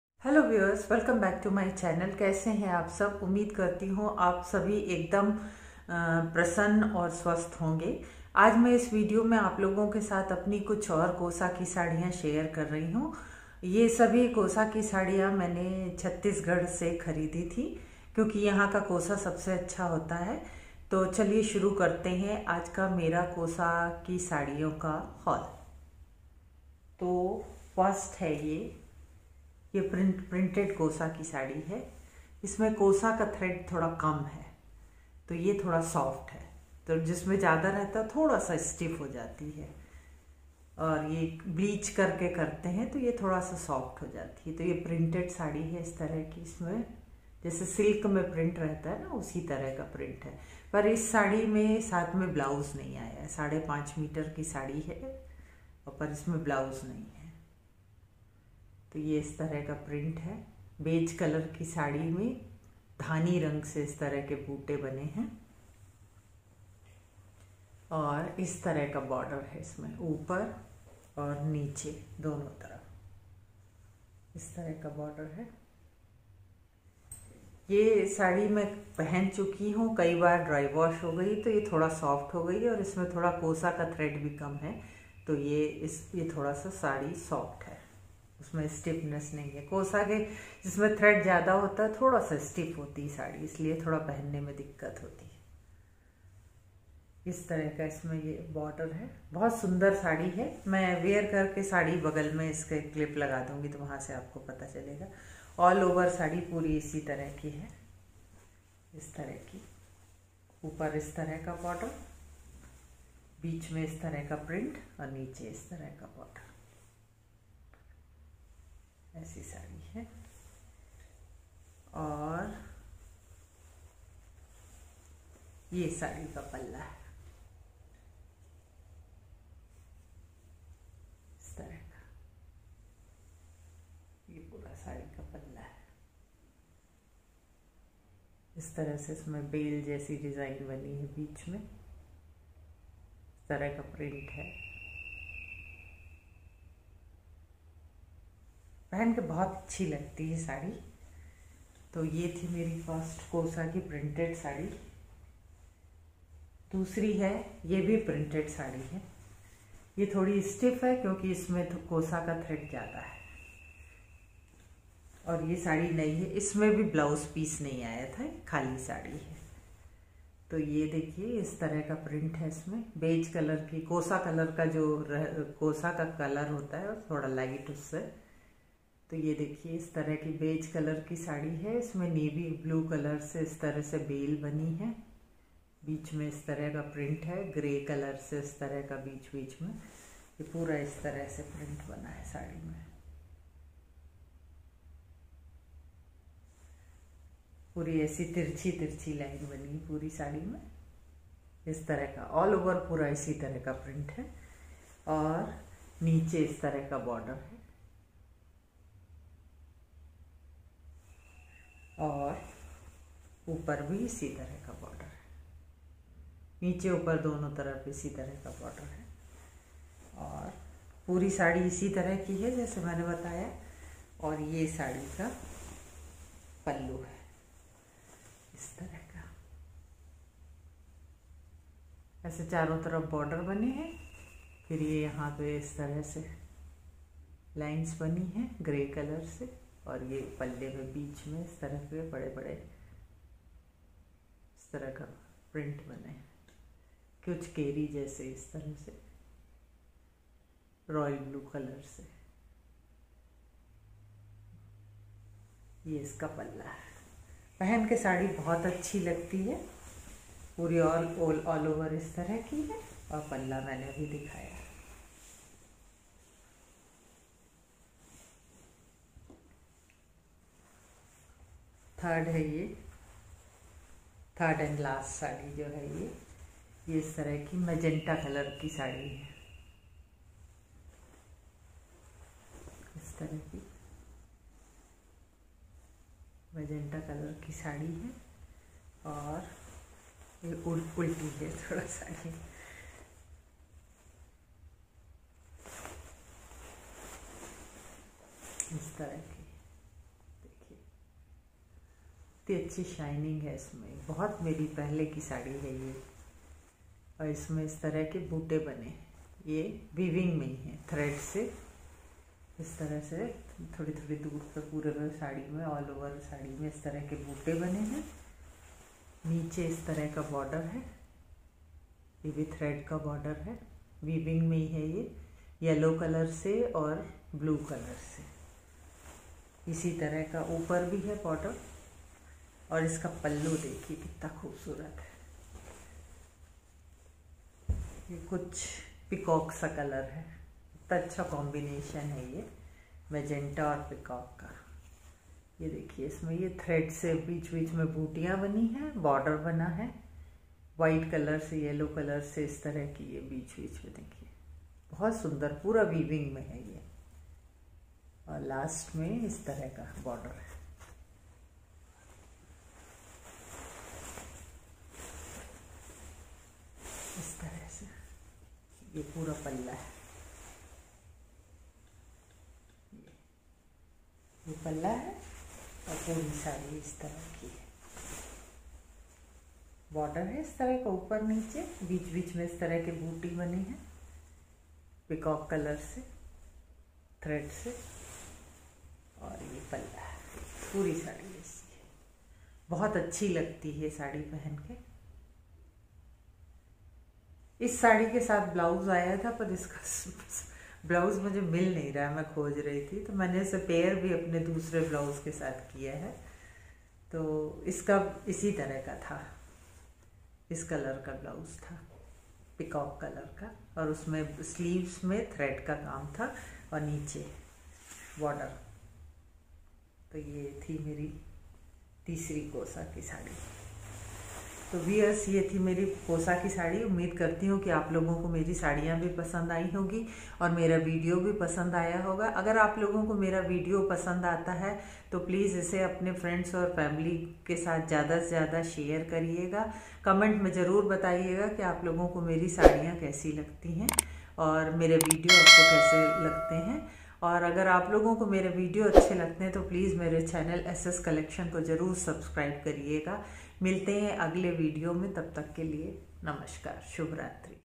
Kosa Saree Haul || Try On Kosa saree Haul || SS. Collection
In today's video I am sharing  some of my kids silk sarees.These saree was purchased from Chattisgarh State.Kosa silk from Chattisgarh is best and world wide famous.